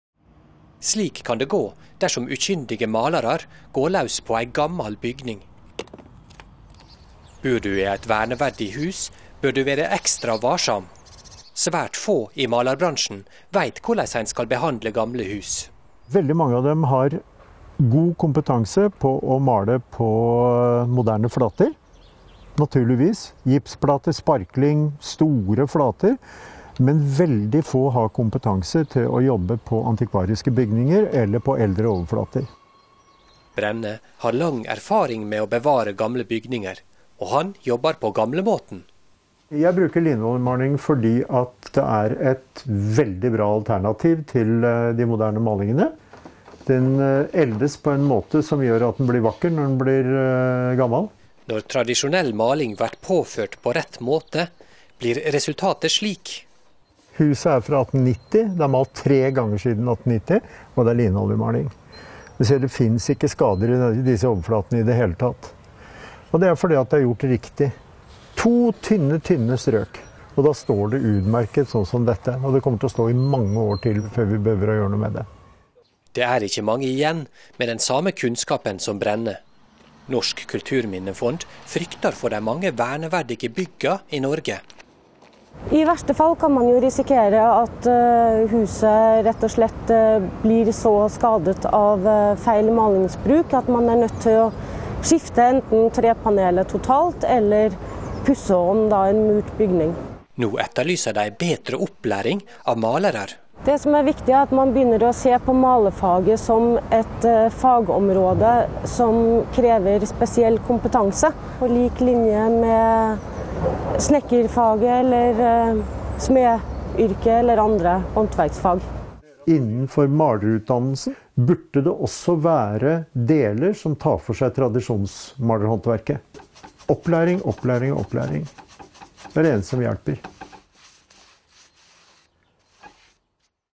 Kunnskapsløse malere ødelegger kulturminner
Malere som ikke vet hva de driver med risikerer å ødelegge gamle og verneverdige bygninger. Det trengs bedre opplæring, mener Norsk Kulturminnefond. (Av Av Rolf Magnus W. Sæther/Newswire)